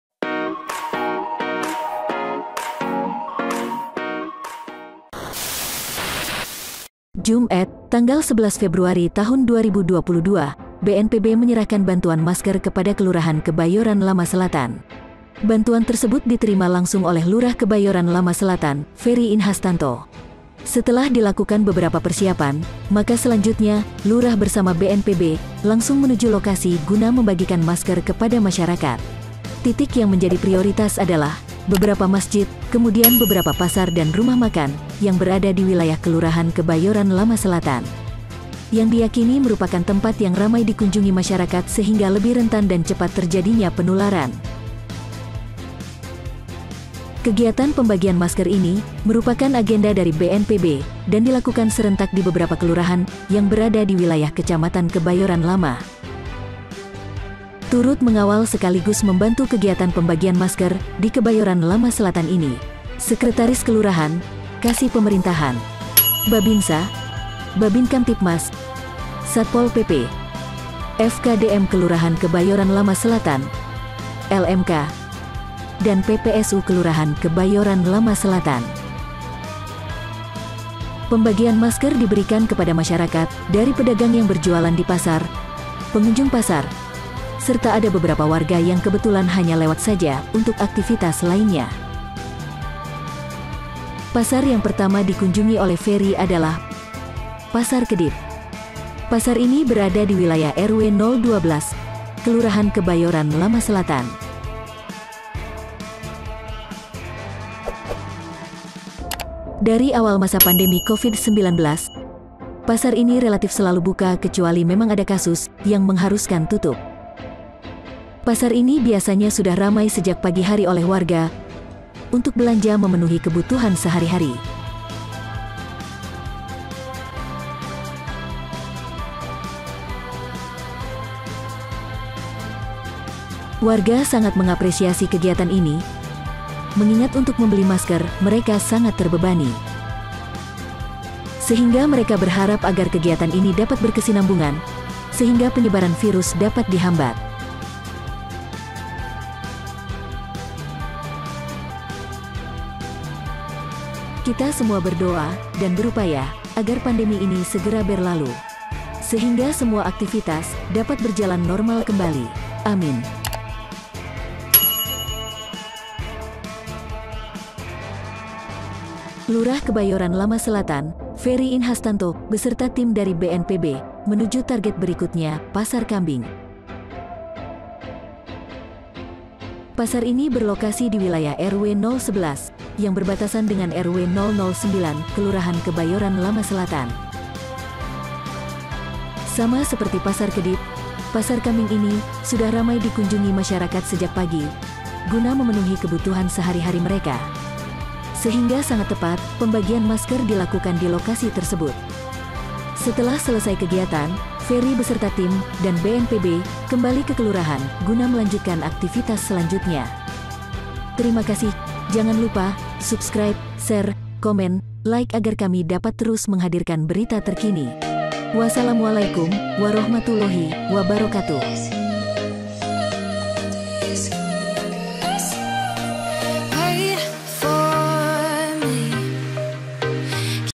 Sweeping dan Pembagian Masker Kelurahan Kebayoran Lama Selatan
Setelah dilakukan beberapa persiapan, maka selanjutnya, Lurah bersama BNPB, langsung menuju lokasi guna melakukan Sweeping dan membagikan masker kepada masyarakat di Kelurahan Kebayoran LAma Selatan.

Titik yang menjadi prioritas adalah, beberapa Masjid, kemudian beberapa pasar dan rumah makan, yang berada di wilayah kelurahan kebayoran lama selatan, yang diyakini merupakan tempat yang ramai dikunjungi masyarakat sehingga lebih rentan dan cepat terjadinya penularan.

Kegiatan pembagian masker ini merupakan agenda dari BNPB, dan dilakukan serentak di beberrapa kelurahan yang berada di wilayah Kecamatn Kebayoran Lama.

Turut mengawal sekaligus membantu kegiatan Sweeping dan pembagian masker di kebayoran lama selatan ini, Babinsa, Bhabinkamtibmas, Satpol PP, FKDM Kelurahan Kebayoran lama selatan, LMK, PPSU Kelurahan Kebayoran LAma Selatan.

Pembagian masker diberikan kepada masyarakat, dari pedagang yang berjualan di Pasar, pengunjung pasar, serta ada beberapa warga yang kebetulan hanya lewat saja, untuk aktifitas lainnya.

Pasar yang pertama yang dikunjungi adalah Pasar kedip.  Pasar ini berada di wilayah RW 012 kelurahan kebayoran lama selatan.

Dari awal masa pandemi covid 19, pasar ini relatif selalu buka kecuali memang ada kasus, yang mengharuskan tutup. Pasar ini biasanya sudah ramai sejak pagi hari oleh warga, untuk belanja kenutuhan sehari-hari.

Warga sangat mengapresiasi kegiatan ini, mengingat untuk membeli masker sangat membebani.  Sehingga mereka berharap agar kegiatan ini dapat berkesianambungan, sehingga penyebaran virus dapat dihambat.

Lurah Kebayoran Lama Selatan, Ferry Inhastanto, beserta tim dari BNPB, menuju target berikutnya, Pasar Kambing. Pasar ini berlokasi di wilayah RW 0 11 Kelurahan Kebayoran Lama Selatan.
Sama seperti pasar kedip, pasar kambing ini, sudah ramai dikunjungi sejak pagi, guna memenuhi kebutuhan sehari-hari mereka.

Setelah Selesai kegiatan, Fery beserta tim, dan BNPB, kembali ke kelurahan, gguna melanjutkan aktifitas selanjutnya.